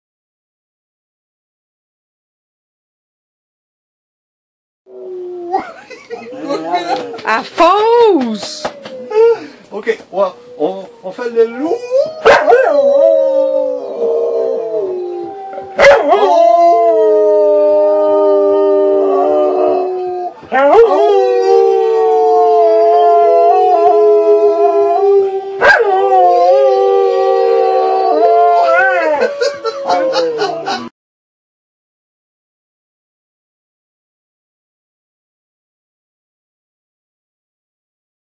American Idogs Shadow my dog, training to howl at the moon like a wolf
As a pro dog trainer, I'm always trying new stuff to teach my dogs. This is my cute rescue dog Shadow a Catahoula Leopard in her third training session where I'm teaching her how to howl at the moon like a wolf.